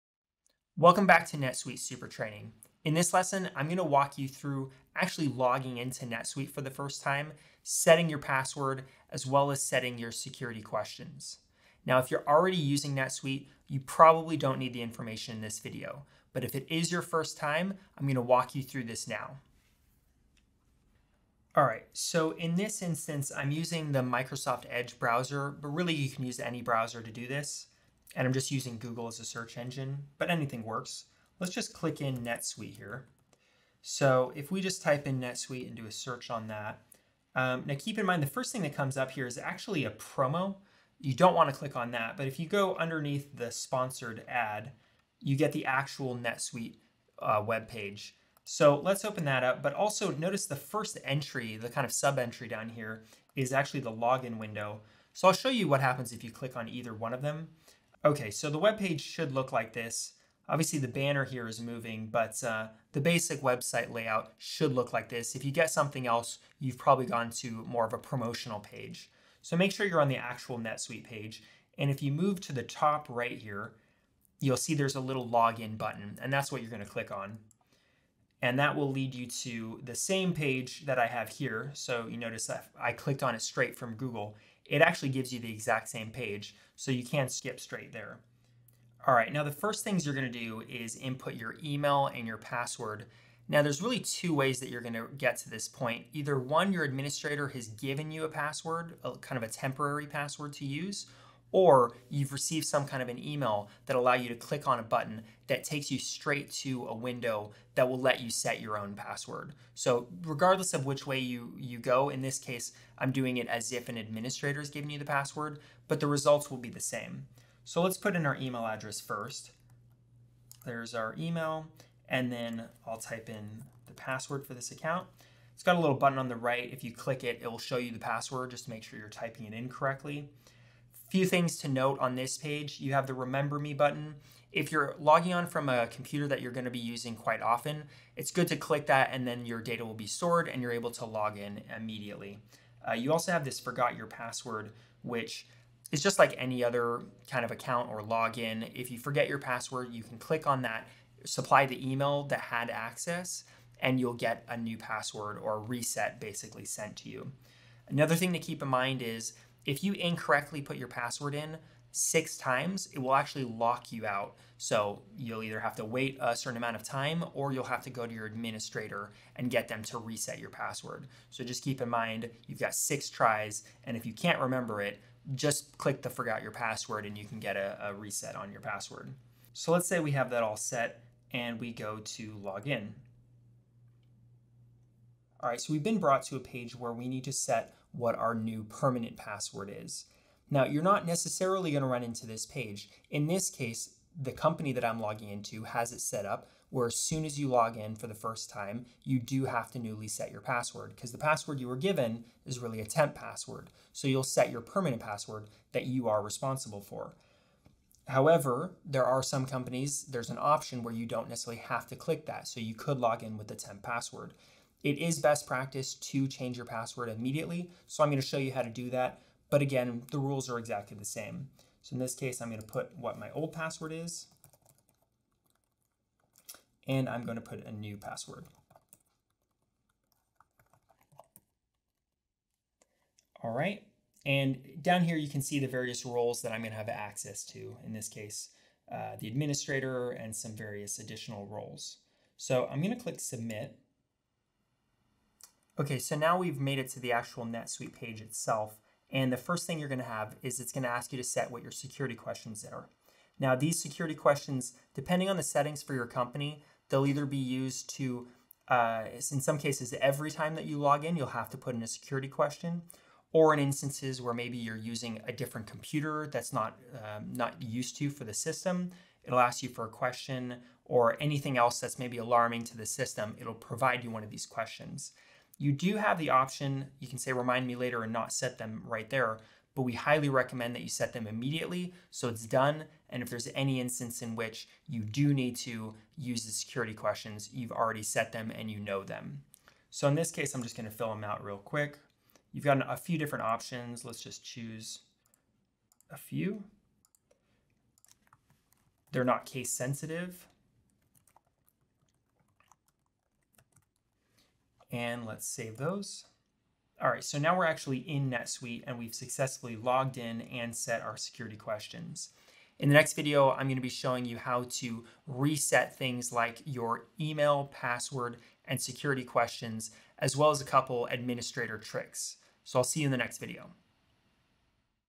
NetSuite Logging in with a Temporary Password
In this lesson, learn how to log into NetSuite for the first time using a temporary password.

These videos are part of a series of NetSuite courses, which you can access

Intro - 00:00
Logging In - 00:19
Setting Password - 03:06
Setting Security Questions - 04:04
Outro - 05:10